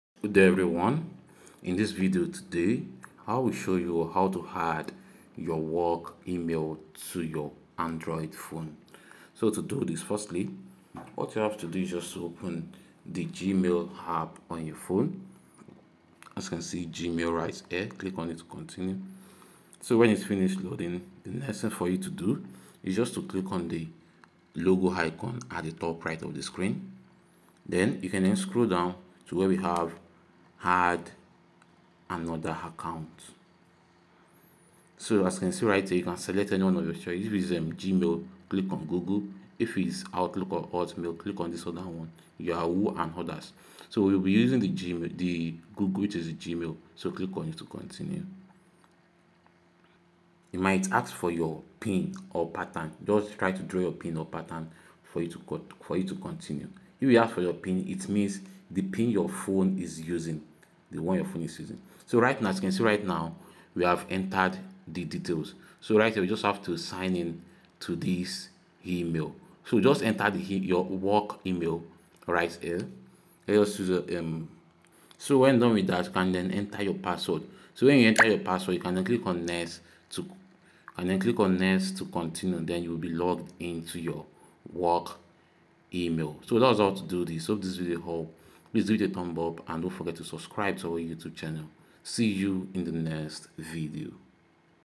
How To Add My Work Email To My Android Phone
In this video, I will show you How To Add My Work Email To My Android Phone.